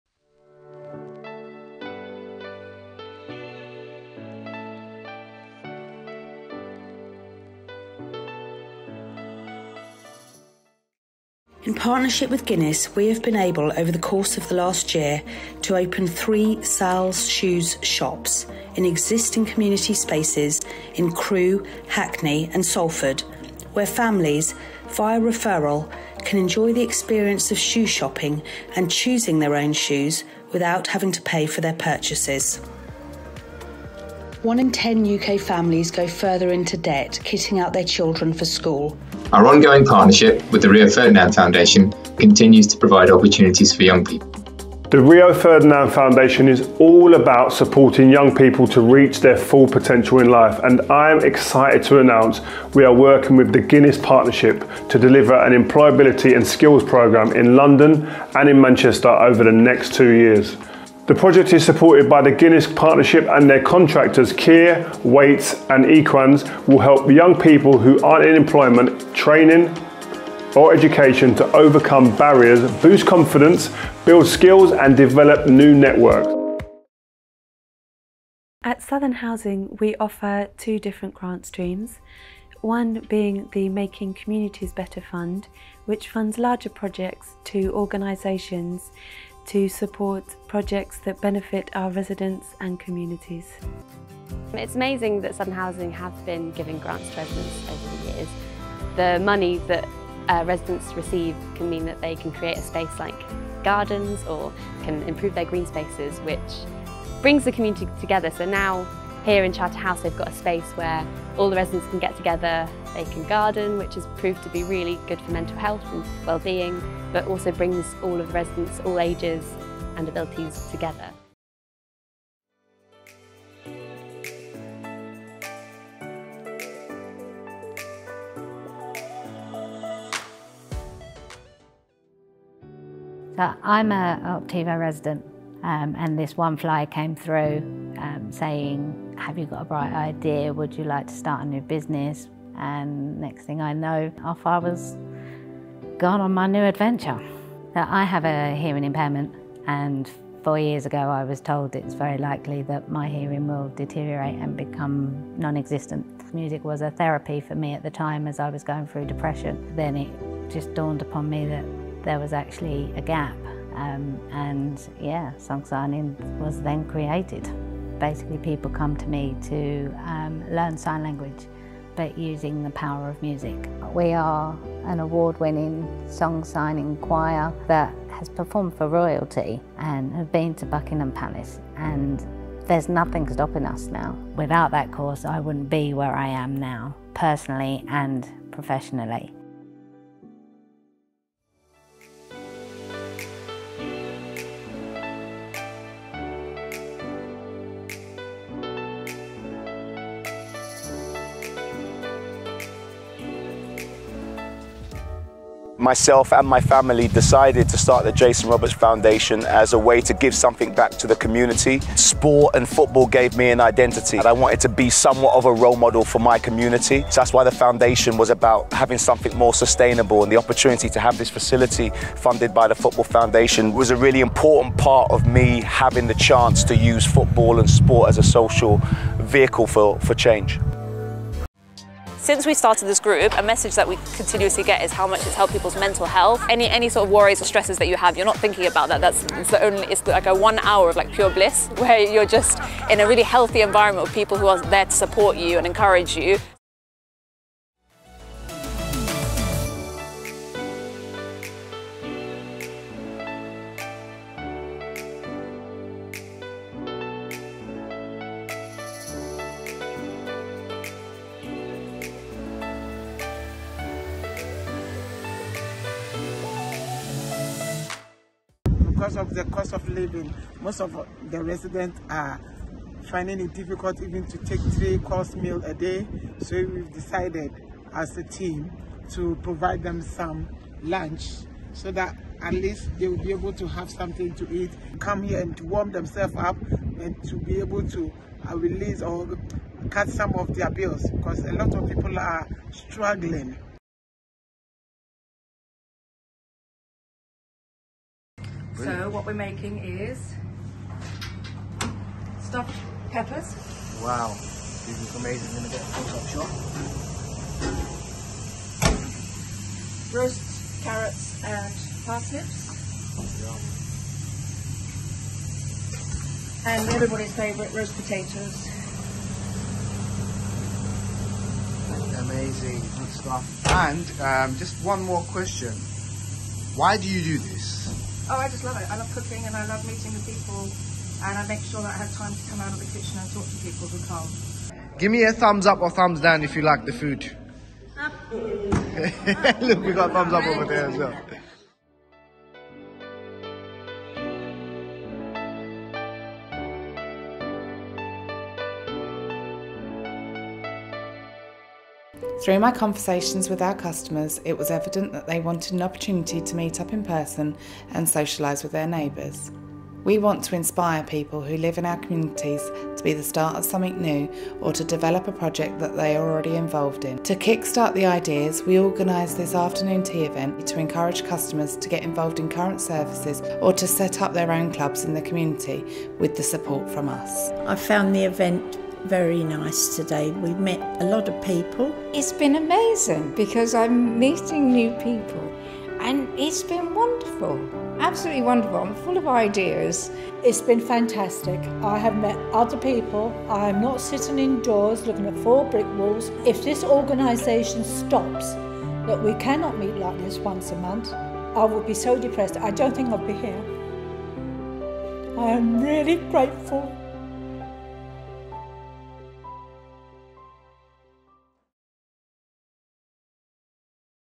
G15 - Support when it matters most
Just some of the ways G15 members are working with residents, communities, and partners to support people when it matters most.